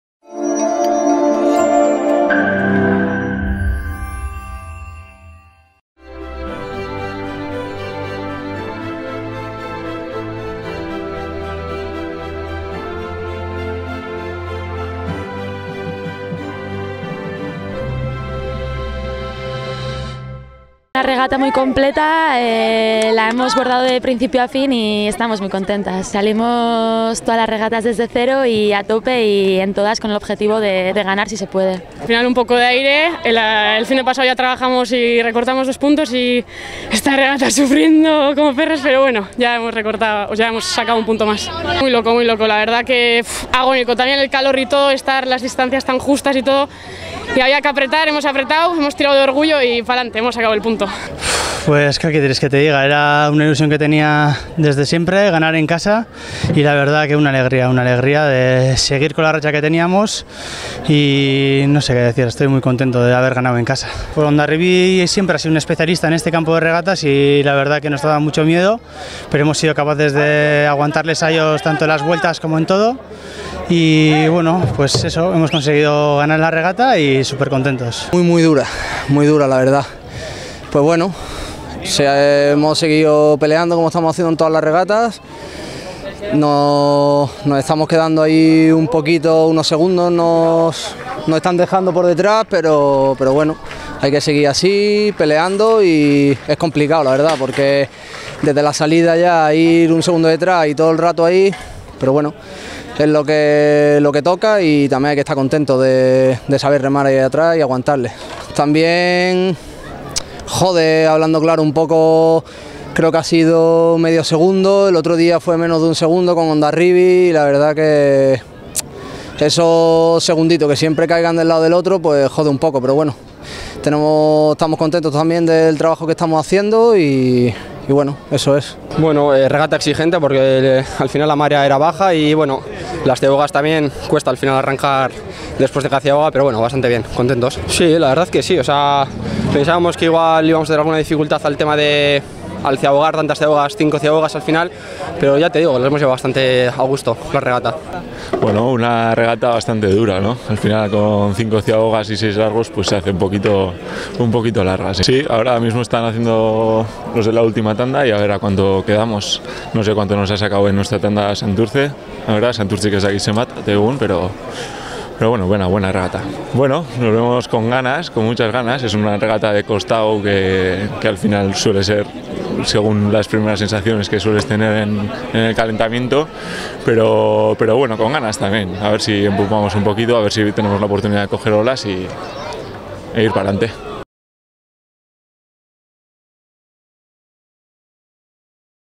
Declaraciones II Bandera ADEGI - Gipuzkoako Foru Aldundiko II. Bandera
Los protagonistas de la II Bandera ADEGI - Gipuzkoako Foru Aldundiko II. Bandera  toman la palabra